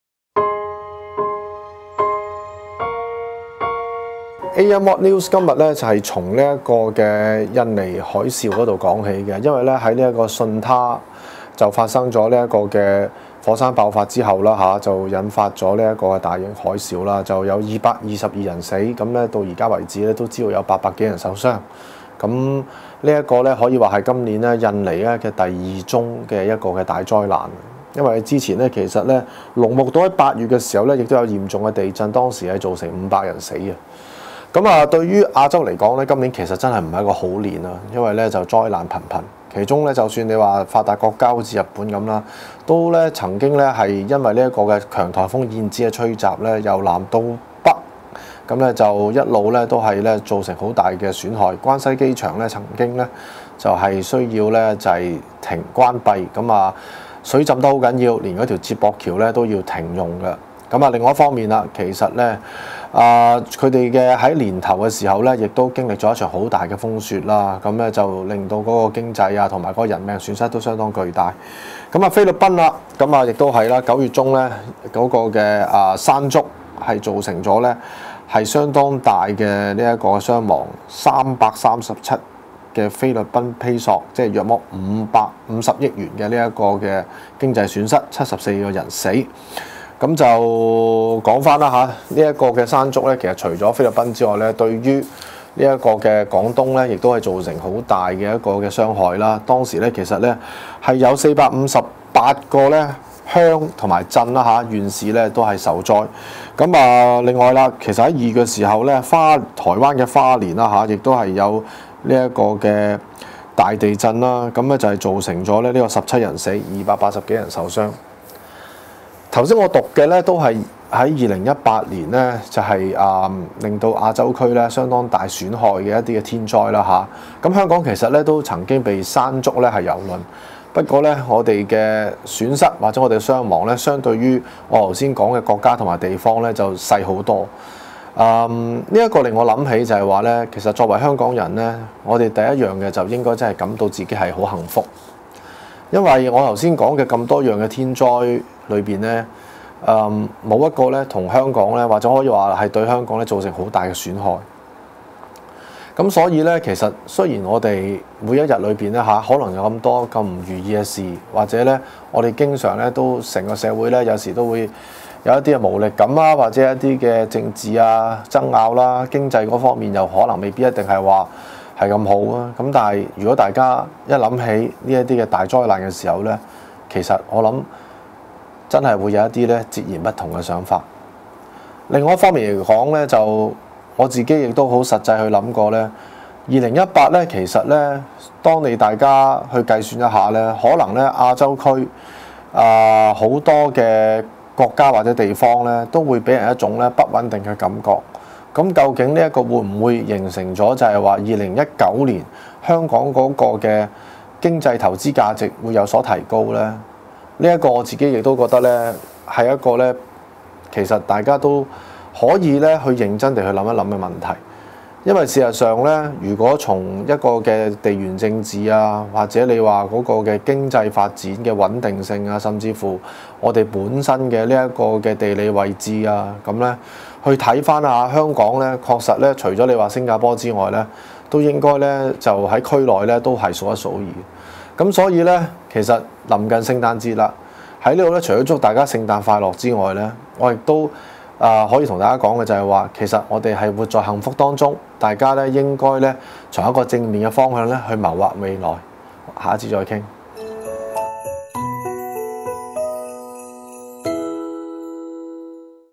【馮振超-What News？】亞洲多國屢遭天災蹂躪 幸福港人應正面謀將來
2018年天災頻生，尤其是亞洲國家地區，日本、印尼、菲律賓及內地等均因為天災而受到不同程度的破壞，而且每每造成人命傷亡。對照香港，就算遭受山竹吹襲，仍能做到「零死亡」，確實非常幸運。香港人，既然已獲得上天眷顧，面對日常生活或計劃將來時，何不採取更積極態度呢？

bmgproductionmusic